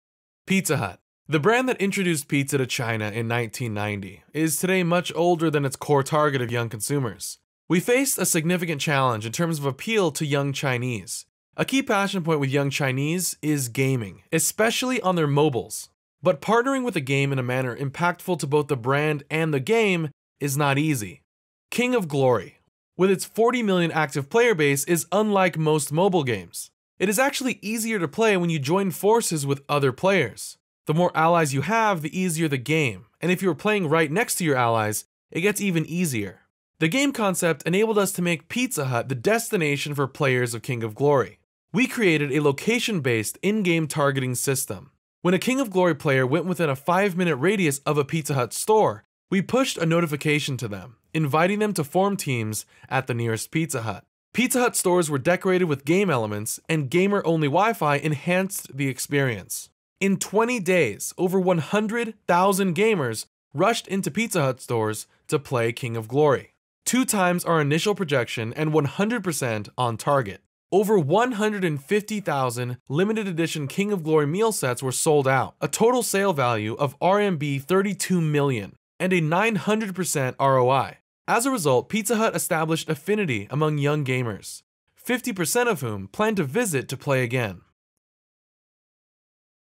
Pizza Hut China - The Social Enabler
Gaming integrations are becoming mainstream in China but impactful gaming integrations based on player insight are few and far between. By understanding how games are played rather than trying to do vanilla ads in games we better the gaming experience and create value for our clients' business